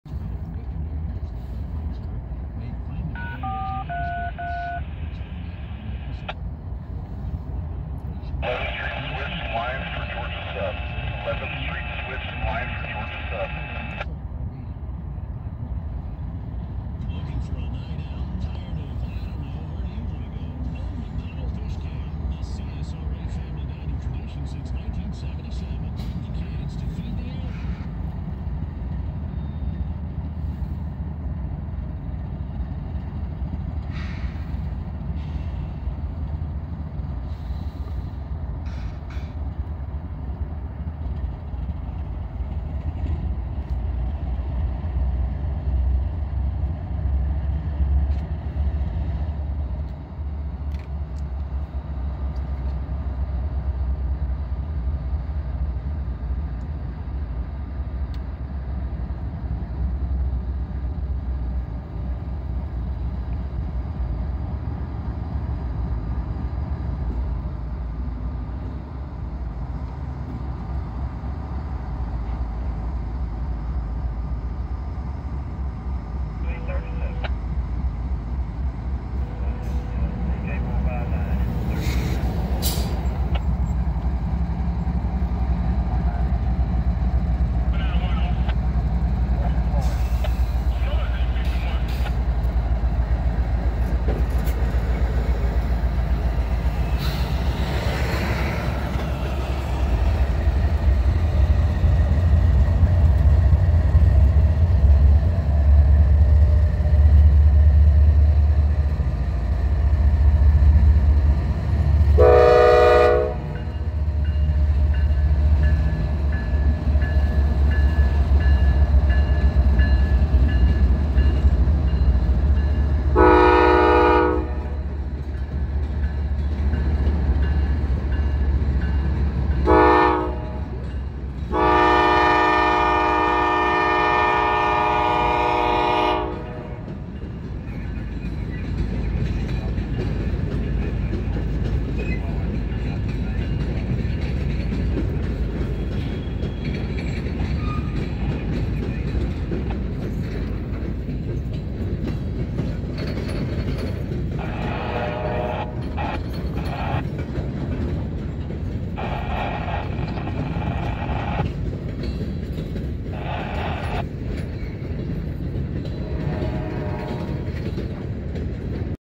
SNIPPET: L837 Passes CSX 4762 Leaving Augusta Yard, Georgia - 11-02-2023
L837 passes CSX 4762 as they leave Augusta Yard for Camak.

CSX 3200
CSX 722

CSX 4762 TIED DOWN